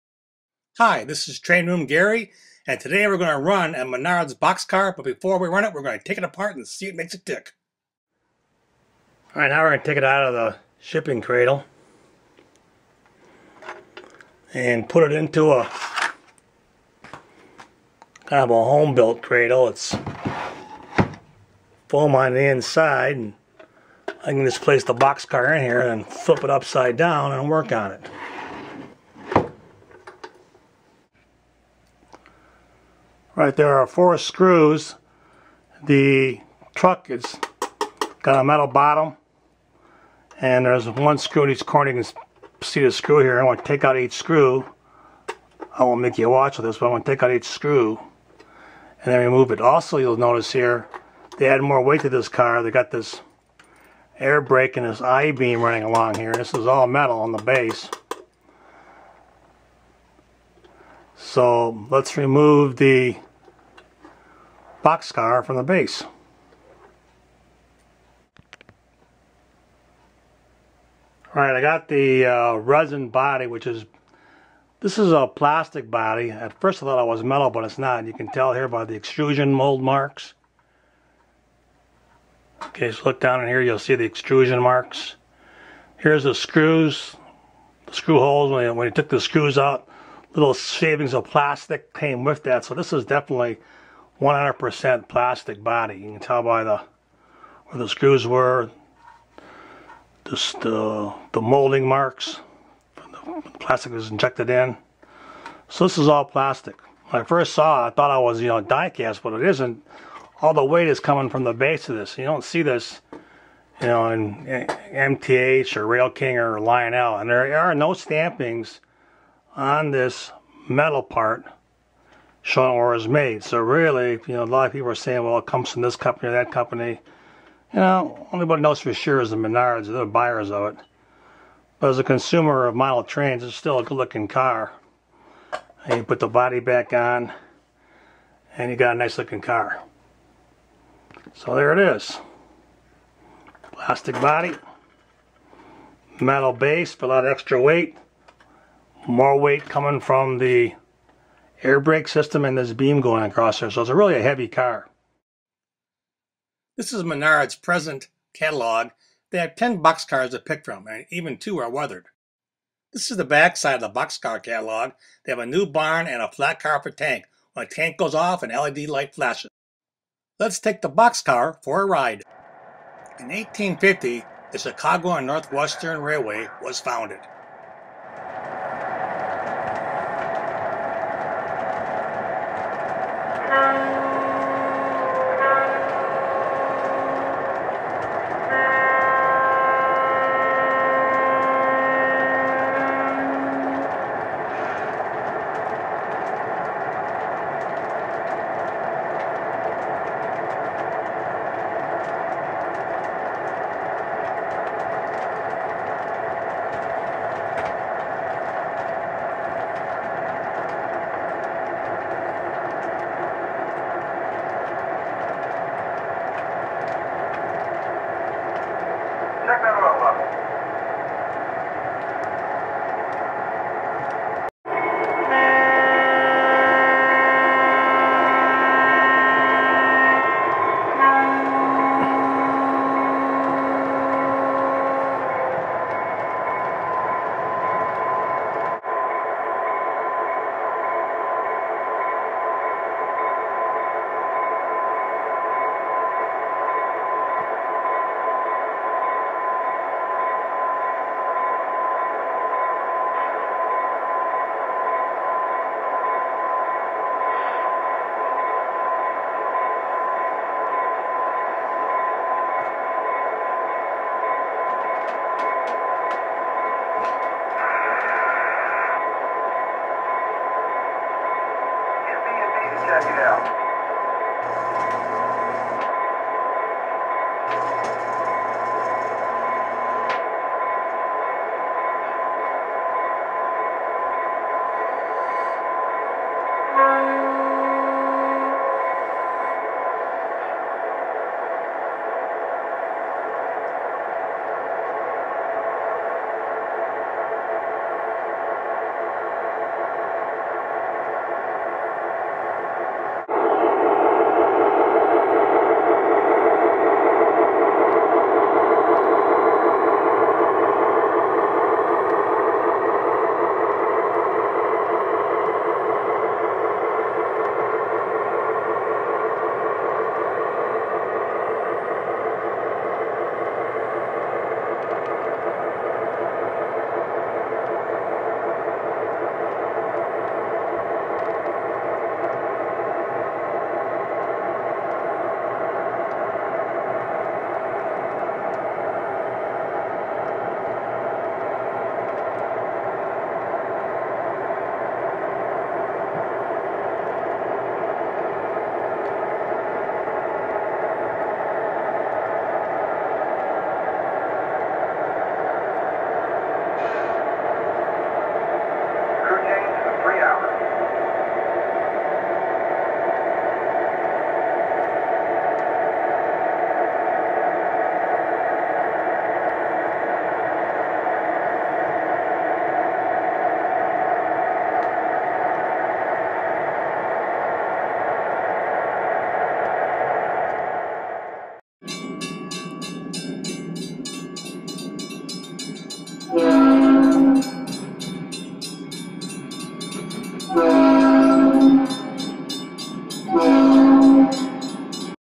MENARDS BOX CAR
MENARDS BOX CAR • In this video I will take apart a MENARDS Box Car to show Model Railroaders, that MENARDS sells a quality box car for $19.99 each. They also sell a weathered version for $24.99 each. They just came out with a U.S. Army Flat Car with a Tank. The tank has animated LED flashing muzzle gun, along with sounds & voices. All MANARDS rolling stock will navigate 0-27 curves.  ▶︎ This video is for ages 14 & up
Cheers from Michigan